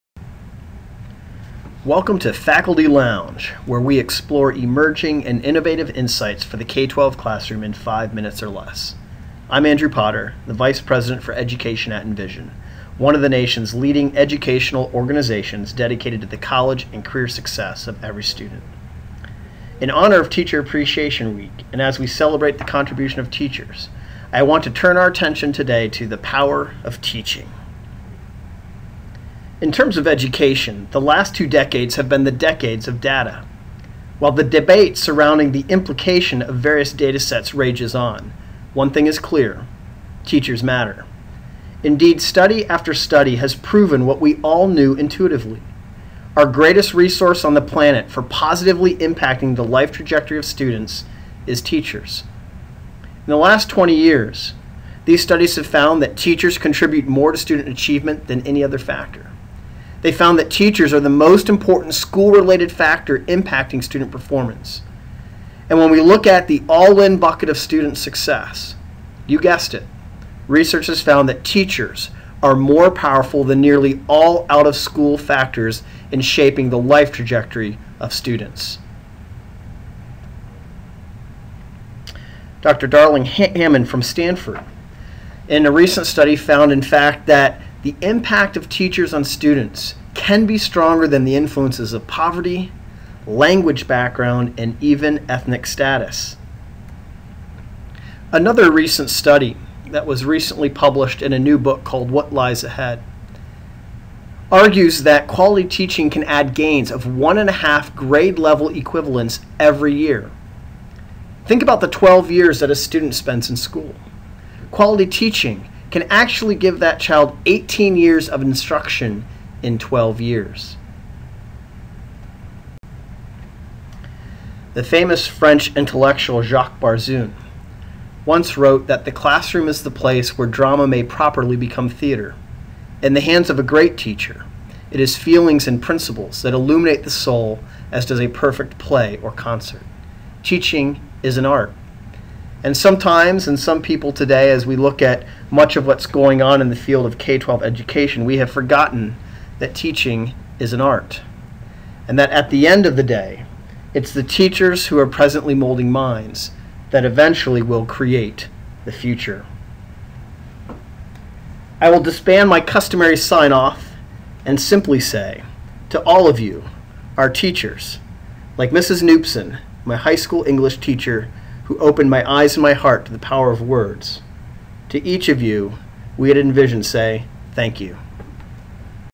Faculty Lounge: Power of Teaching
In this special Faculty Lounge video blog, we salute teachers and the many ways they positively impact young minds during Teachers Appreciation Week. for future Faculty Lounge updates.